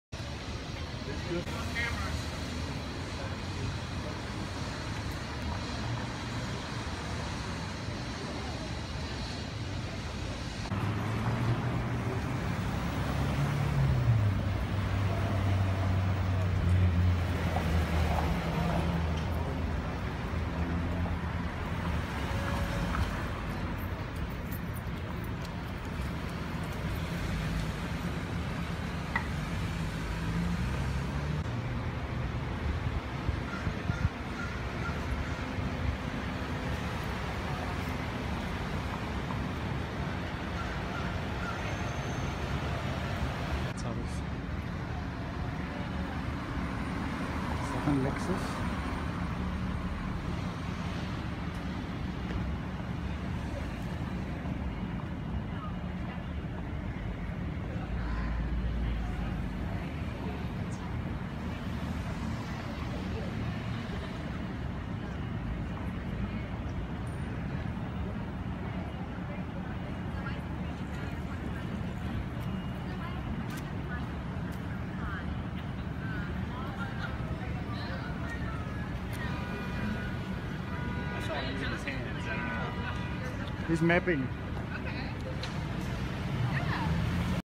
Unknown autonomous Lexus in Palo Alto
This Lexus with autonomous car sensors from an unknown entity drove around the University Avenue and neighboring blocks in Palo Alto on Tuesday, May 23rd, 2017